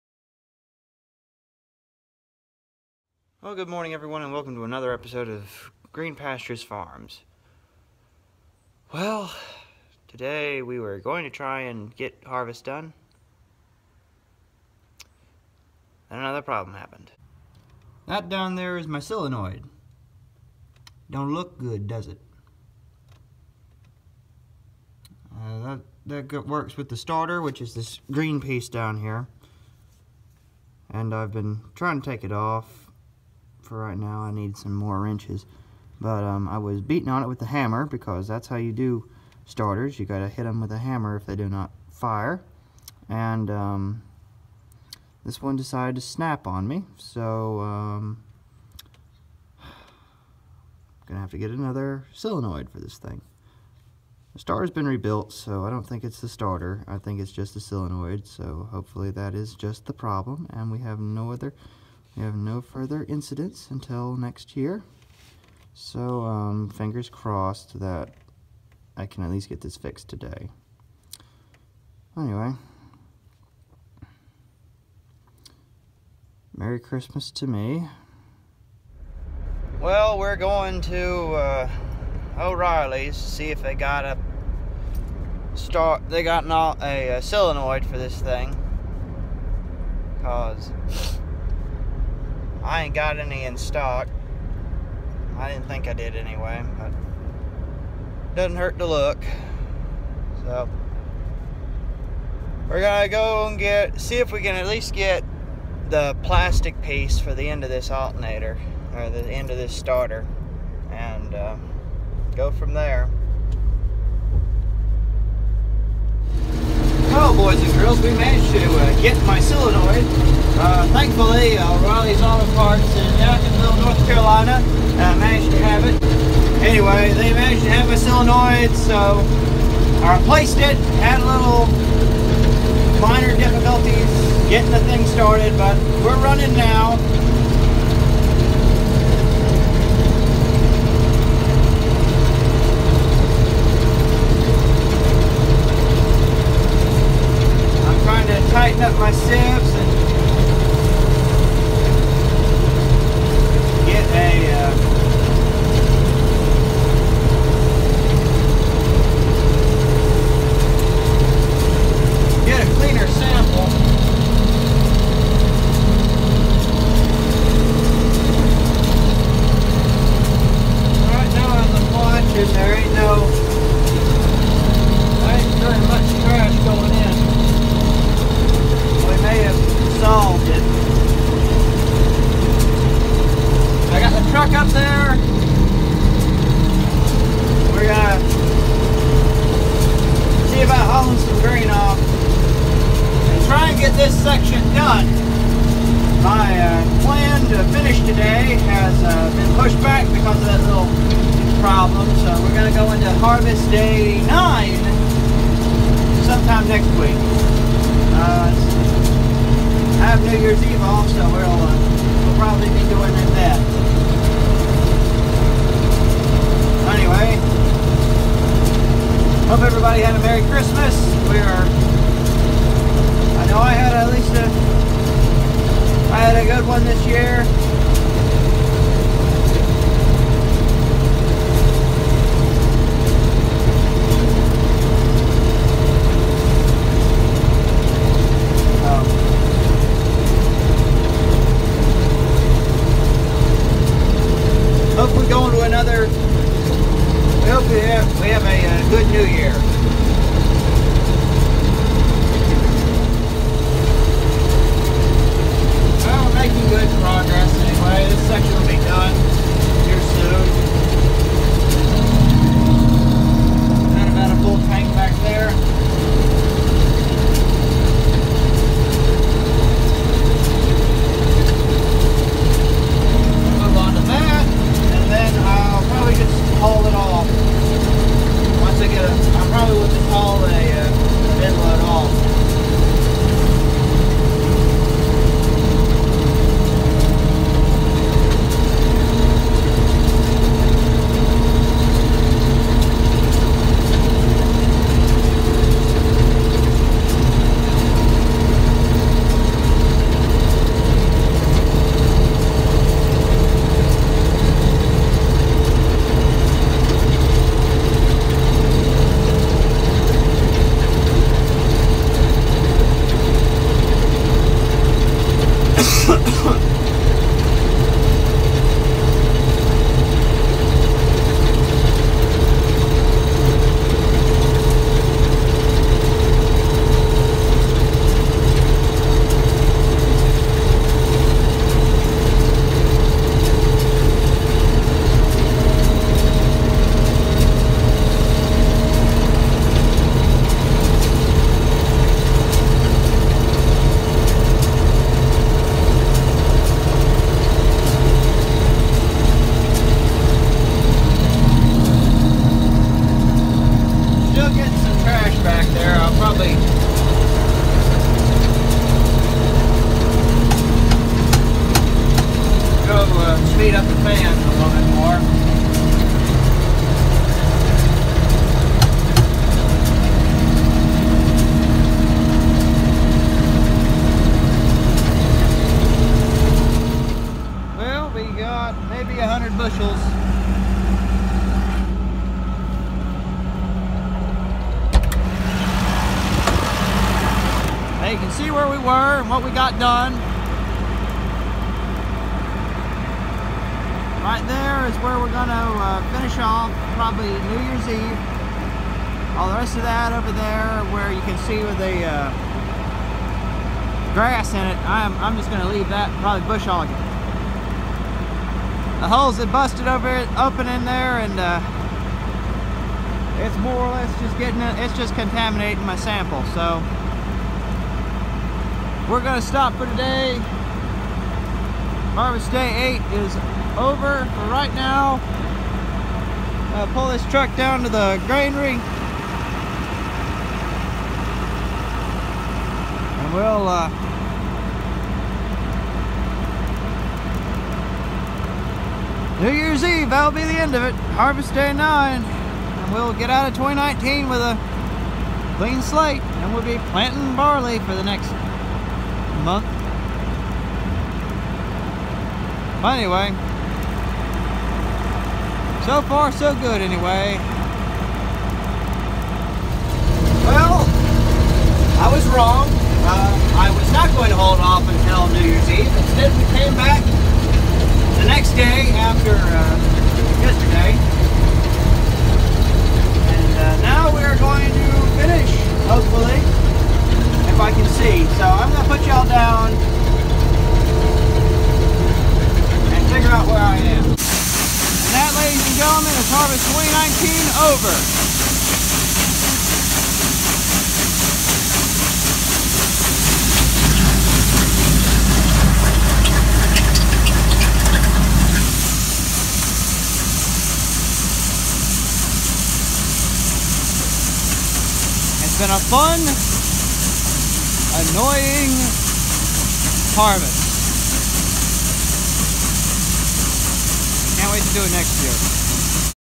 Harvest Day 8 & 9 2019
In today’s video I combined the last two days of harvest into one video. Harvest is done!